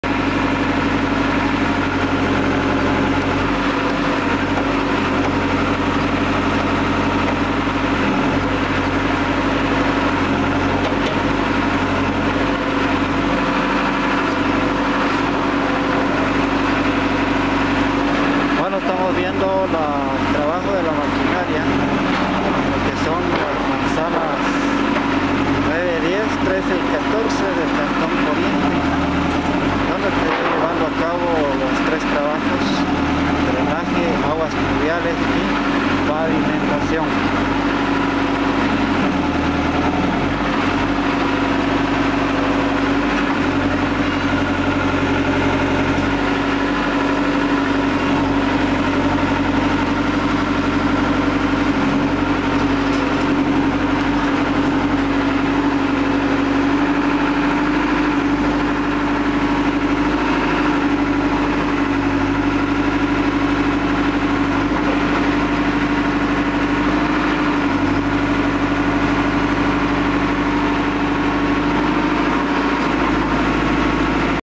Avances proyecto Cantón Poniente
Patzún, Avances proyecto Cantón Poniente para instalación de drenaje y aguas pluviales, manzanas 9-10 y 13-14